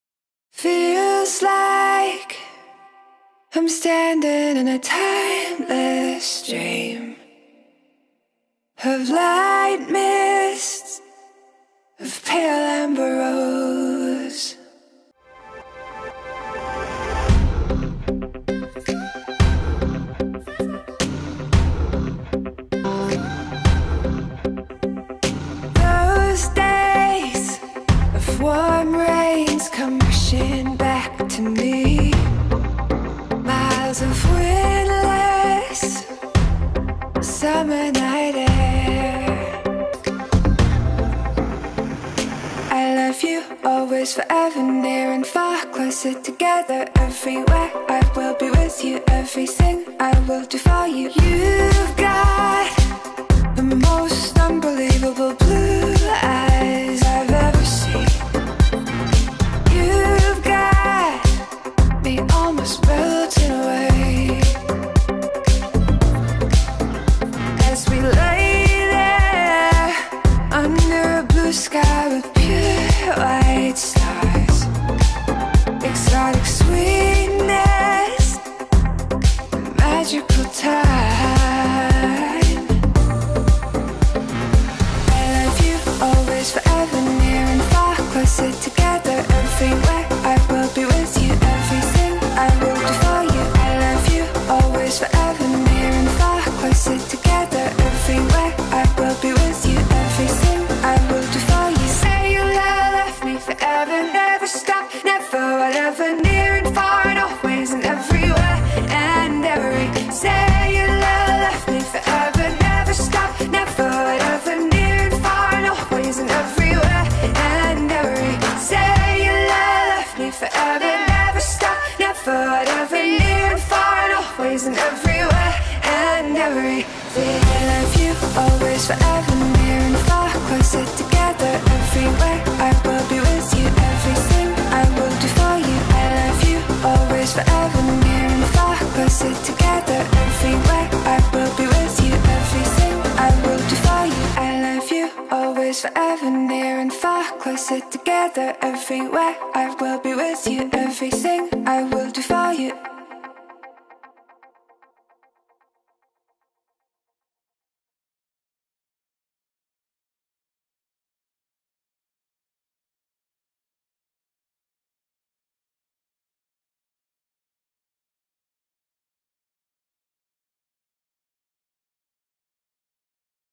The Westwood SunDancers present, "Always Forever" the Virtual Edition
Performed by the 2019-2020 SunDancers
Choreographed by Shelby Kelly